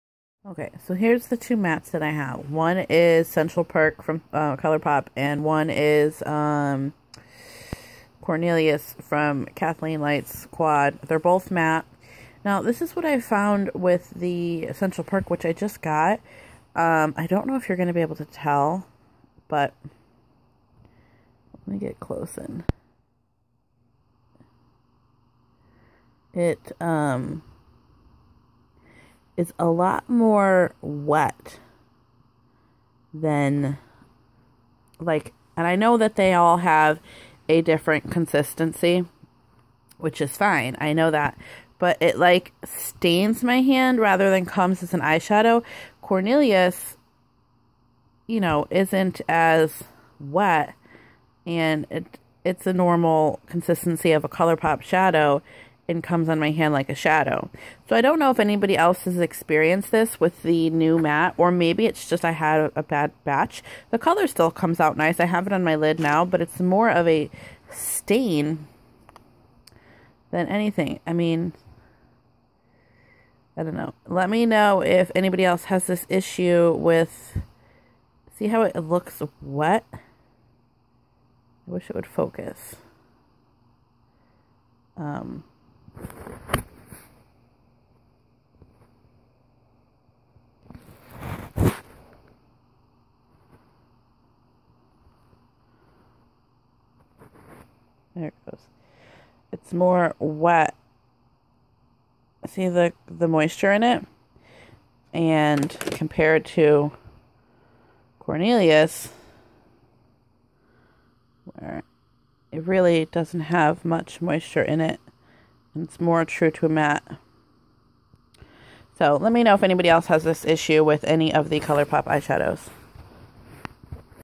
Colourpop matte issues **update**
I love colourpop sooo much but this one shadow may be an issue.. anyone else have this issue
 ***AWESOME CUSTOMER SERVICE, SENT ME A NEW ONE***

Central perk matte finish.